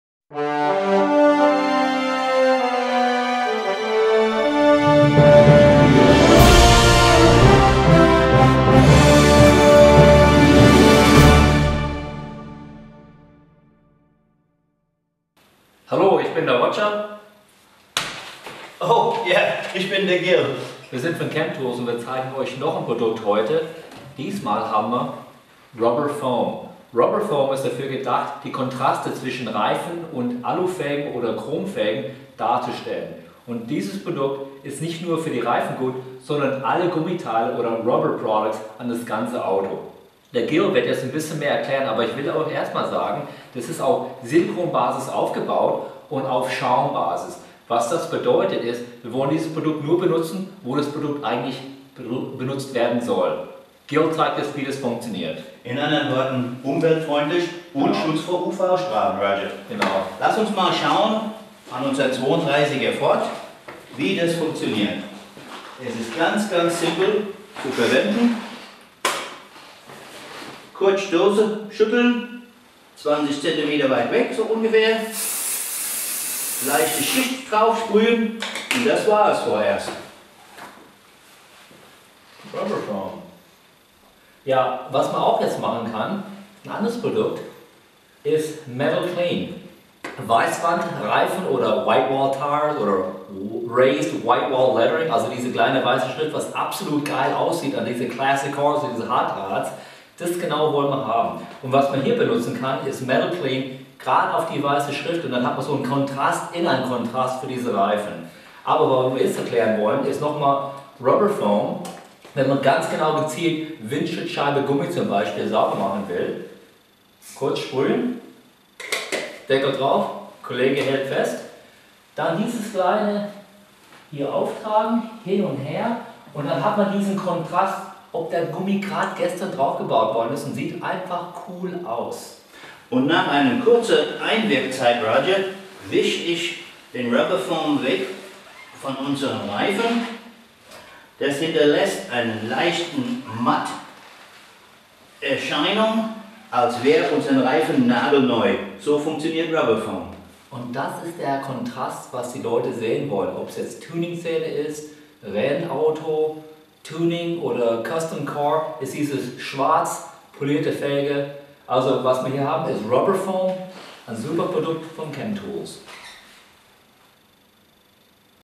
Chem-Tools finest automotive care - RubberFoam - German
RUBBER FOAM
Tiefgehender Gummi- und Kunststoff-Pflegeschaum

Entdeckt bei einer Reaktion unterschiedlicher Rezepturen haben wir einen Kunststoffschaum mit ausgezeichneten Eigenschaften entwickelt, der beanspruchten Oberflächen wertvolle Pflegestoffe, lebendige Farben und einen langanhaltenden Schutz zurück gibt. Nach dem Schütteln der Dose sprühen Sie den haftstarken Spezialschaum mit einem Abstand von ca. 20 cm direkt auf die Oberfläche (z.B. Reifen, Stoßstangen) oder tragen diesen mit einem Tuch gezielt auf. Nach einer kurzen Einwirkzeit reiben Sie ihn für ein gleichmäßiges Ergebnis wieder ab. Der integrierte, langanhaltende Schutz eignet sich wegen einer Rutschgefahr nicht für Lenkräder, Laufflächen, Bremsen oder ähnliches.